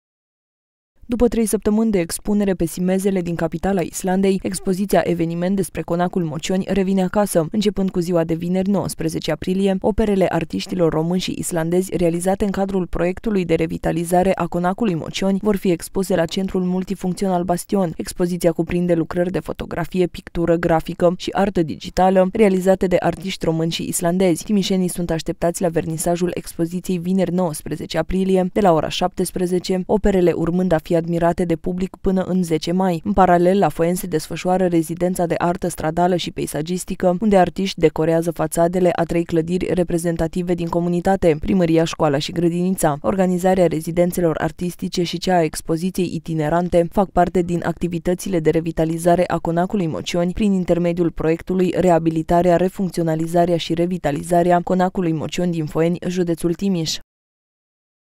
Conacul Mocioni, expoziție dedicată la Timișoara și artă stradală la Foeni
După trei săptămâni de expunere pe simezele din capitala Islandei, expoziția eveniment despre Conacul Mocioni revine acasă. Începând cu ziua de vineri, 19 aprilie, operele artiștilor români și islandezi realizate în cadrul proiectului de revitalizare a Conacului Mocioni vor fi expuse la Centrul Multifuncțional Bastion. Expoziția cuprinde lucrări de fotografie, pictură, grafică și artă digitală realizate de artiști români și islandezi. Timișenii sunt așteptați la vernisajul expoziției vineri 19 aprilie, de la ora 17, operele urmând a fi admirate de public până în 10 mai.